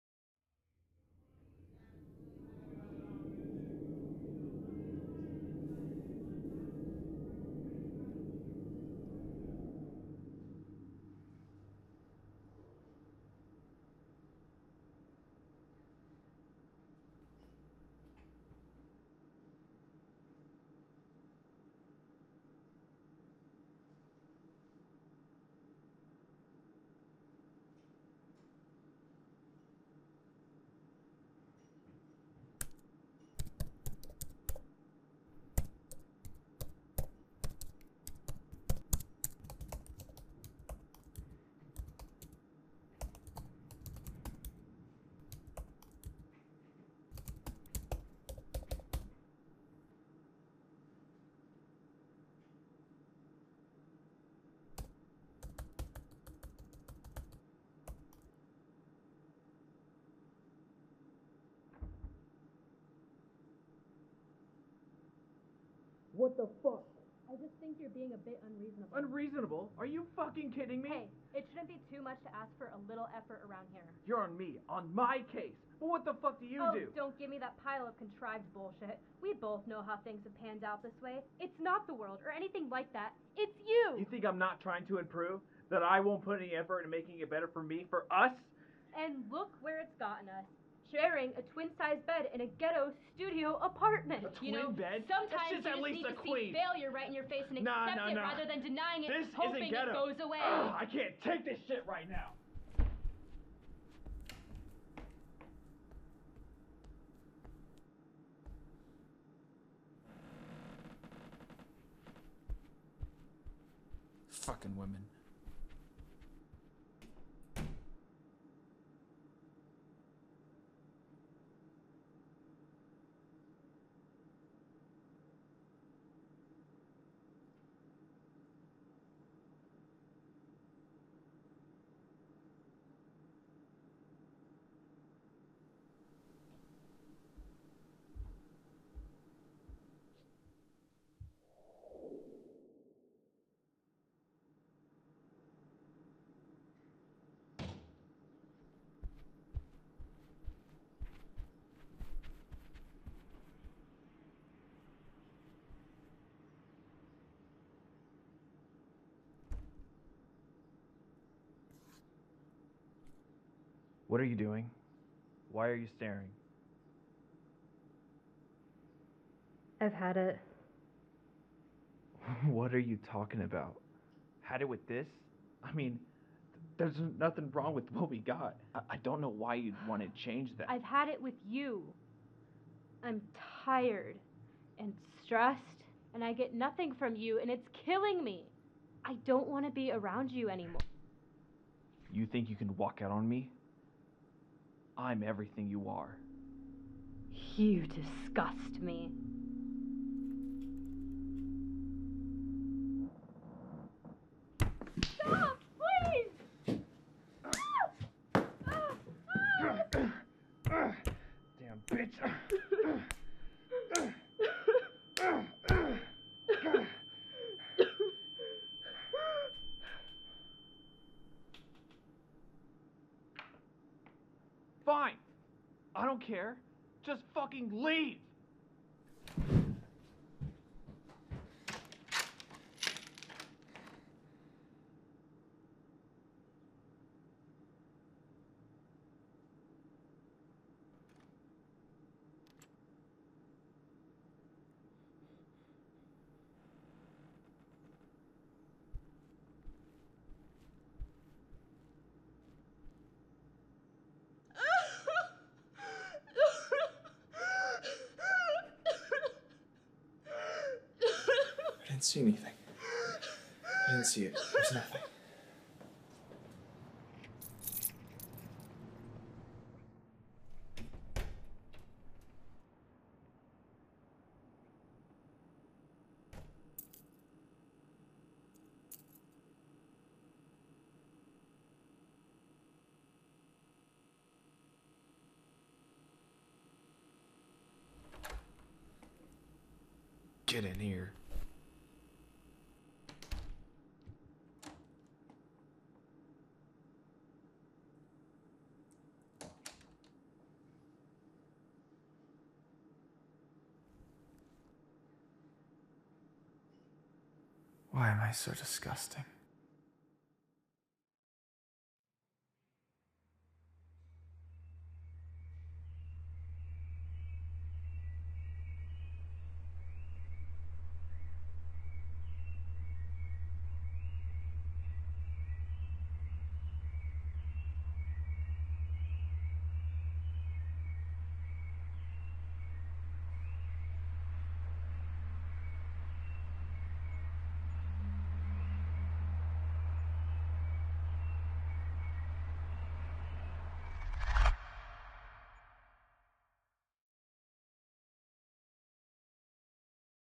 What Muffles Between Our Walls - A Student Short about Domestic Violence (4K)
Synopsis:
A self-loather overhears an argument between two of his neighbors and becomes idly intrigued. However, when it turns into something more serious, he is left with a choice: either to be complicit in it or take a stand against it.

HEY! I'm working on projects such as these all the time!